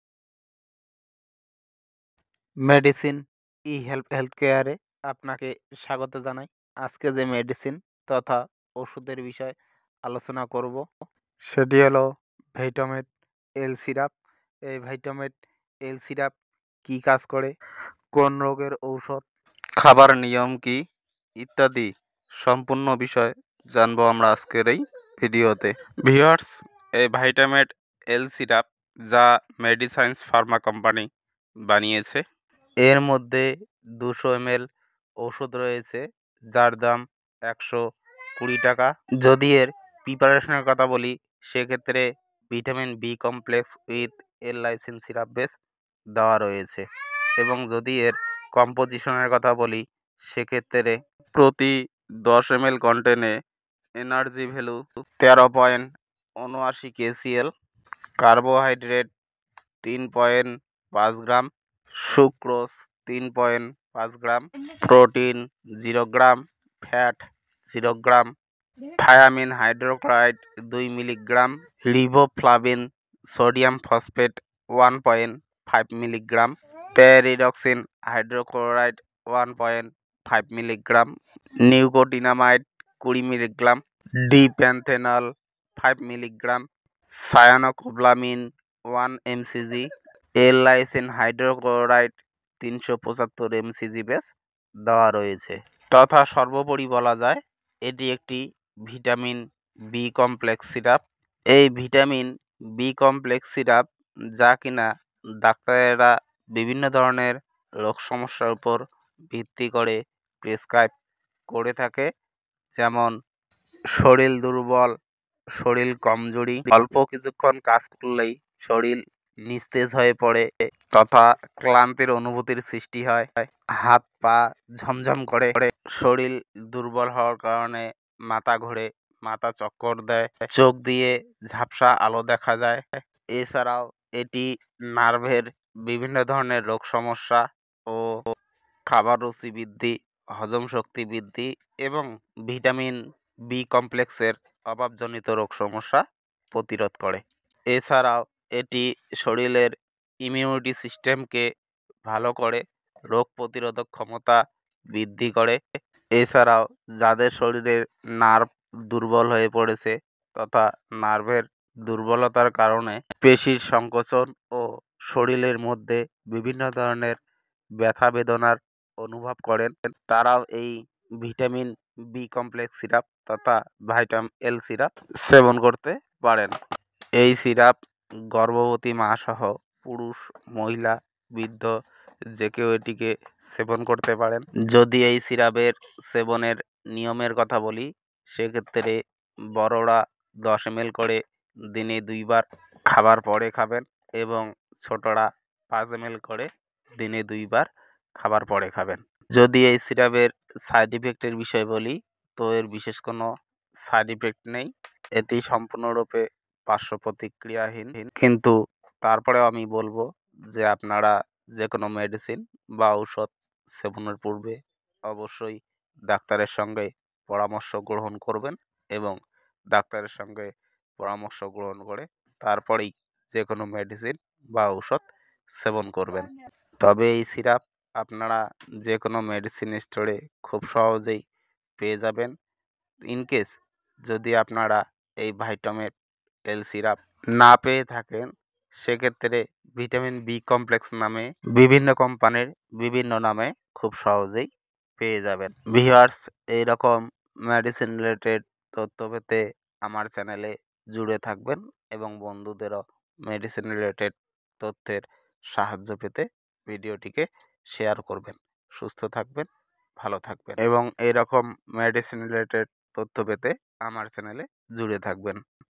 Vitamin B Complex With L-lysine Syrup কি, কি কাজ করে সম্পূর্ণ তথ্য Vitomed L Syrup
Eliminates disease problems caused by lack of vitamin B, improves food taste and improves digestion. Prevents nerve diseases by boosting immunity